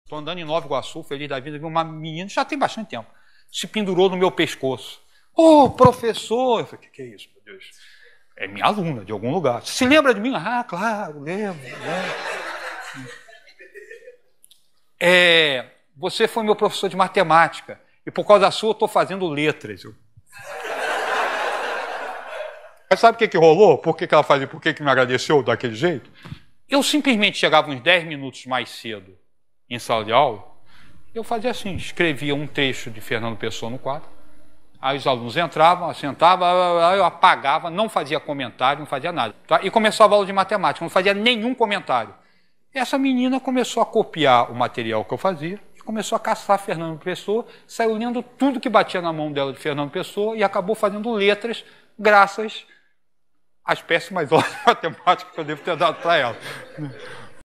POR CAUSA DAS MINHAS AULAS, ELA FEZ LETRAS | Ledo Vaccaro
PAPMEM - Julho de 2017 - Números racionais, irracionais, reais - Prof. Ledo Vaccaro